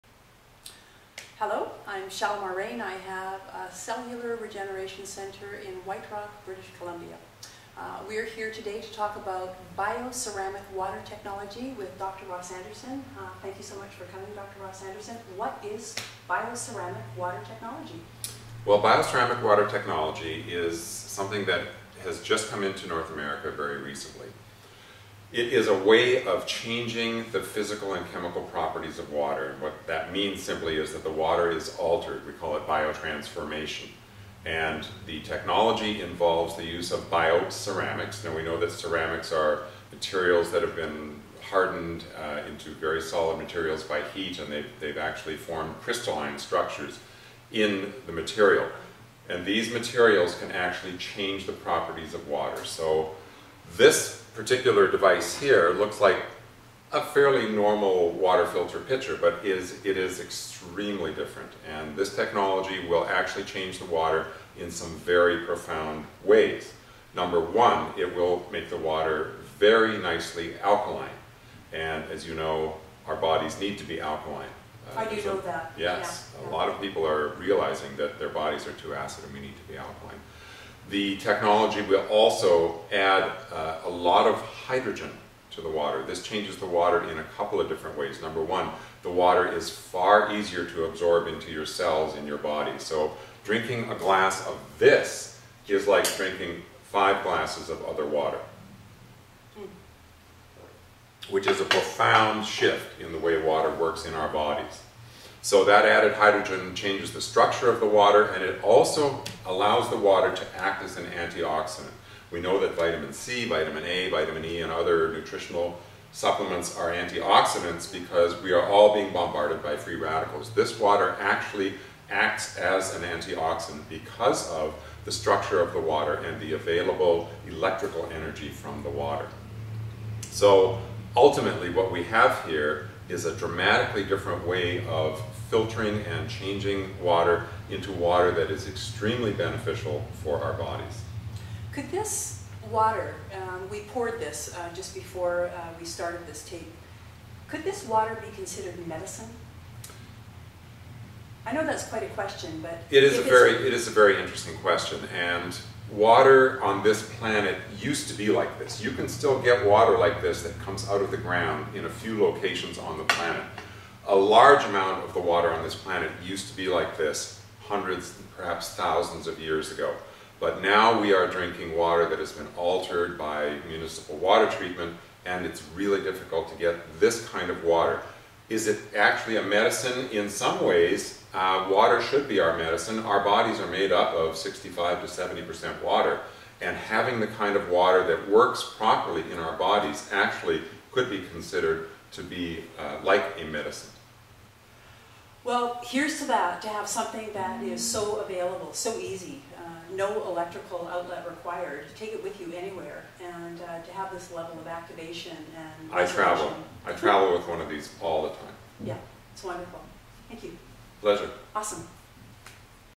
Dr Ross Andersen ND DC - Bio-Ceramic Water Technology Explained
Bio-Ceramics Water Tebchnology is a new technology to North America. Bio-Ceramics change the properties of water in profound ways, making it alkaline, adds hydrogen for increased absorption, and acts as an anti-oxidant.  This is a dramatically different way of filtering and changing normal water into water that is extremely beneficial for our bodies.